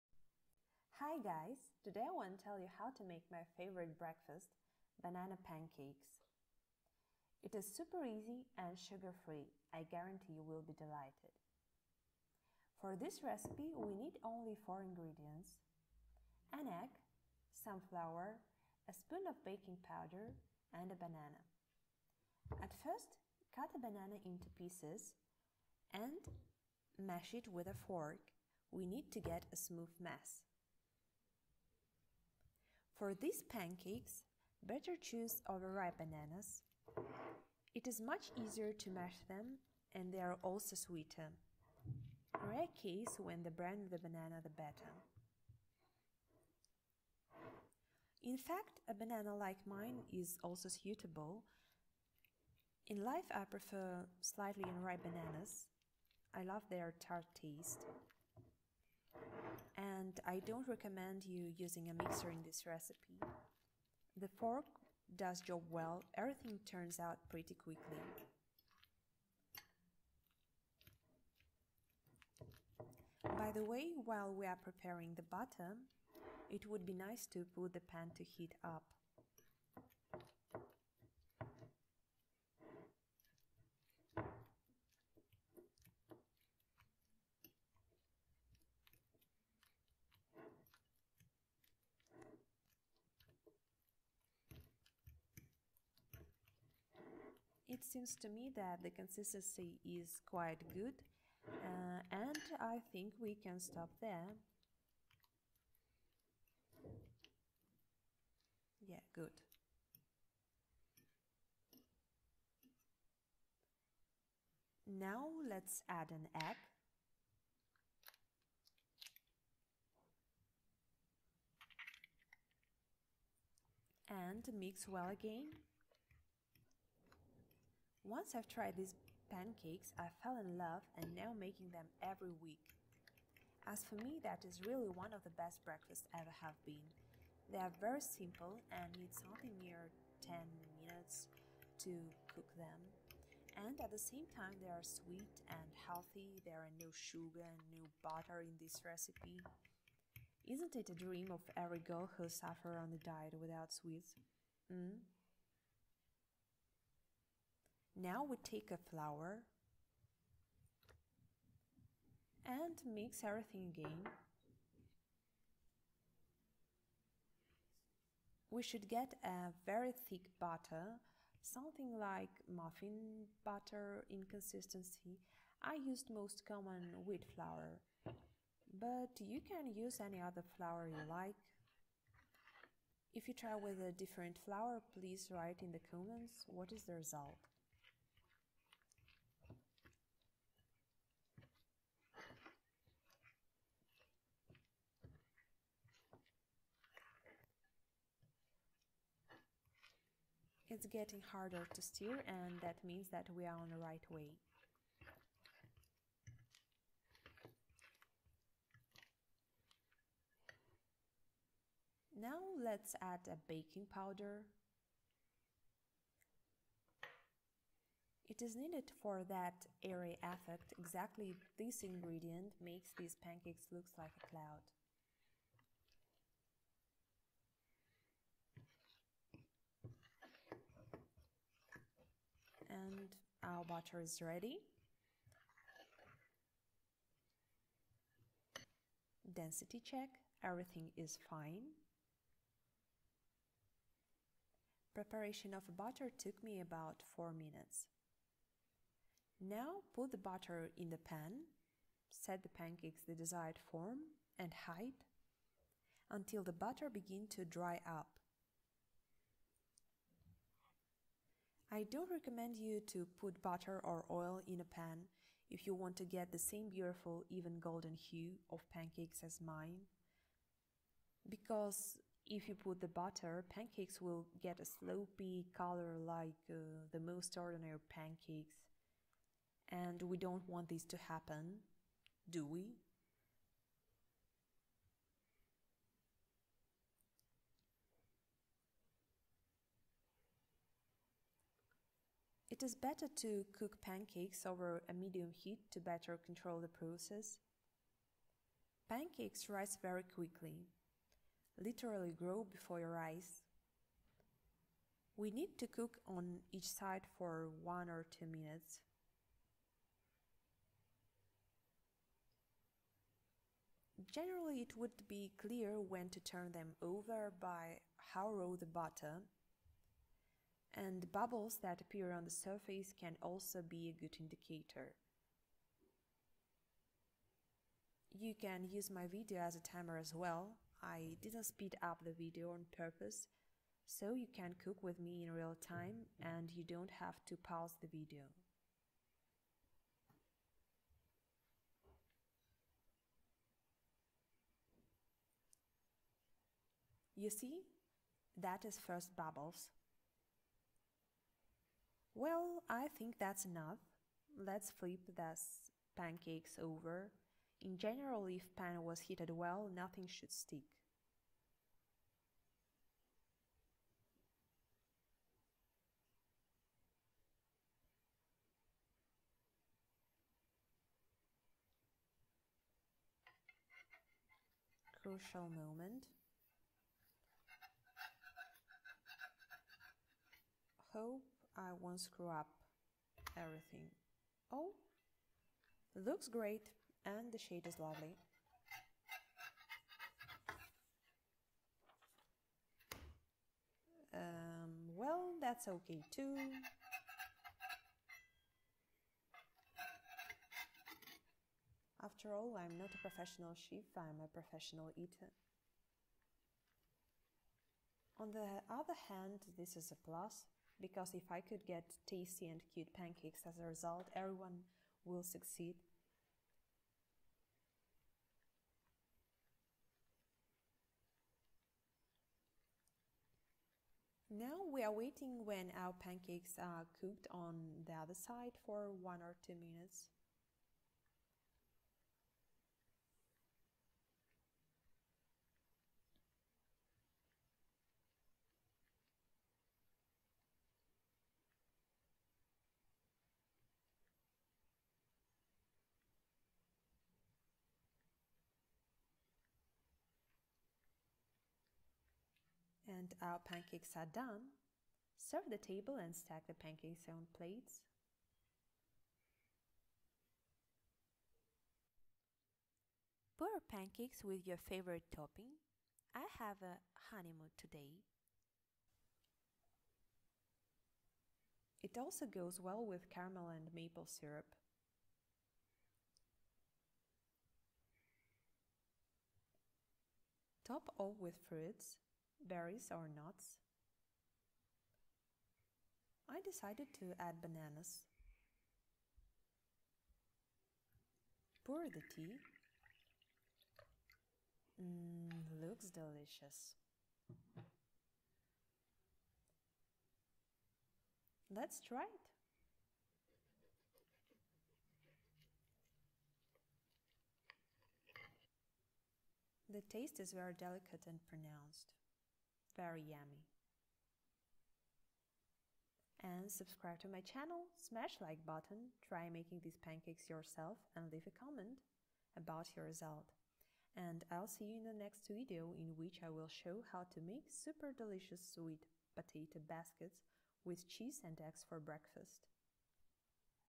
Banana pancakes [No sugar! No milk! No butter! No oven - 4 ingredients only]
In this video you would learn how to make delicious banana pancakes without sugar and butter using only 4 ingredients.

⏲ Whole time of preparation and cooking 10 minutes.

🥞 For 6 pancakes 🥞

Ingredients:
🍌  Banana        1
🥚   Egg               1
🍚  Flour       3-4 tbsp. (depends from size of banana)

Optional:
🍯  honey (or other toppings, for example caramel or maple syrup)
🍌 banana slices (or other fruits, nuts or berries)

You can find more healthy and easy recipes on my channel

Bon appetite!